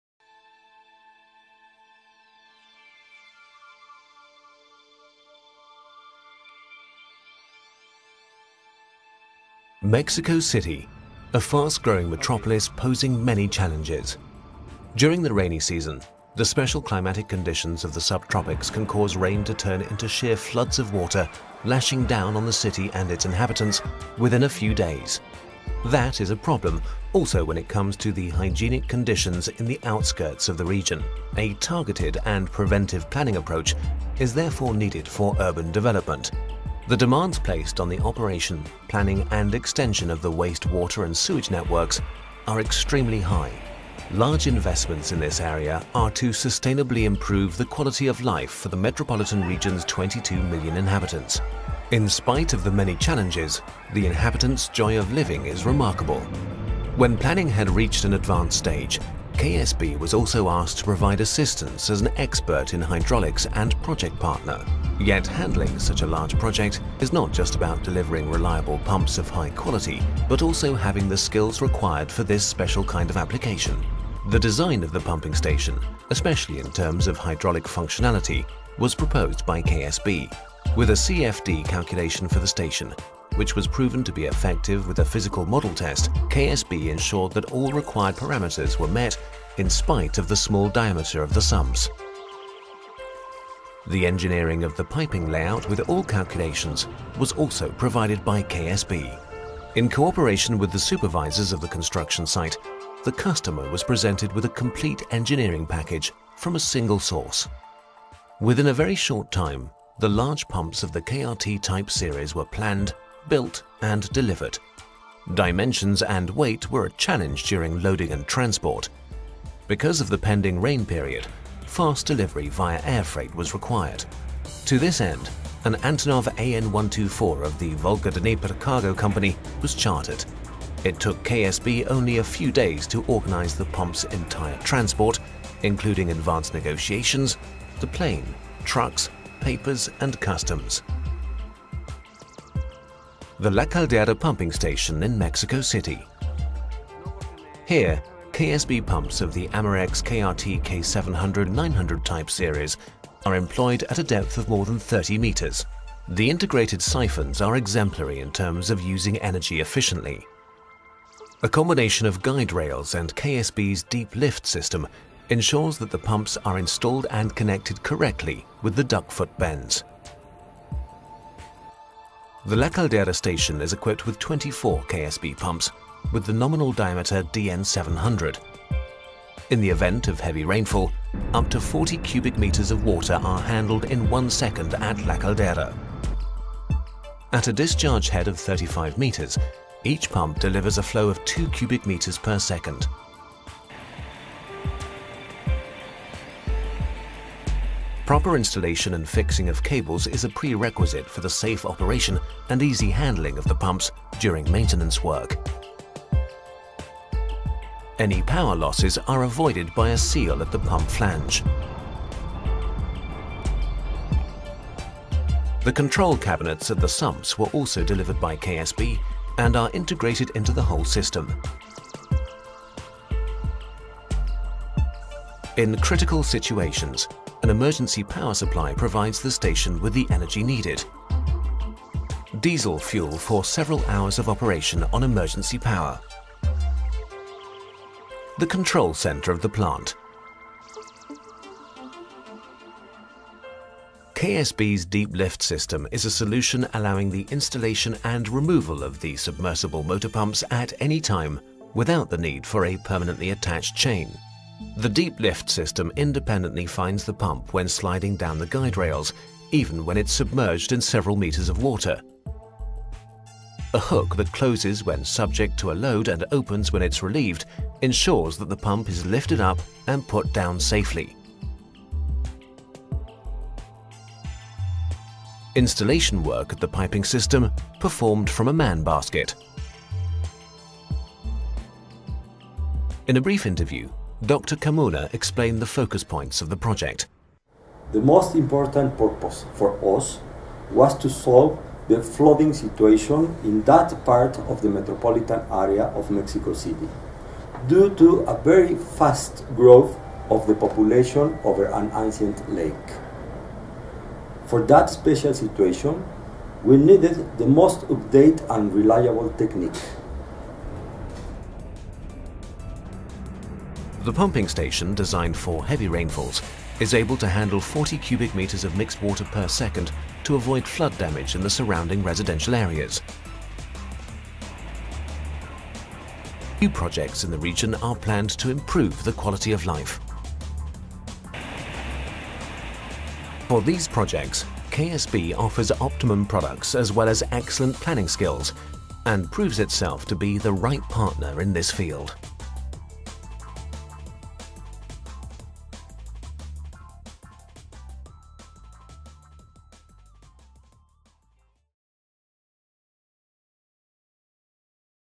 KSB Reference La Caldera, Mexico – Flood control
Mexico City is situated on a plateau at an altitude of about 2,200 m above sea level. Due to the geographical location, natural rainwater run-off is very limited, which made uncontrolled flooding inevitable. In order to provide the city's inhabitants with long-term flood control, CONAGUA, Mexico's national commission for water and waste water, initiated the country's largest and most significant infrastructure construction at that time: the La Caldera pump station.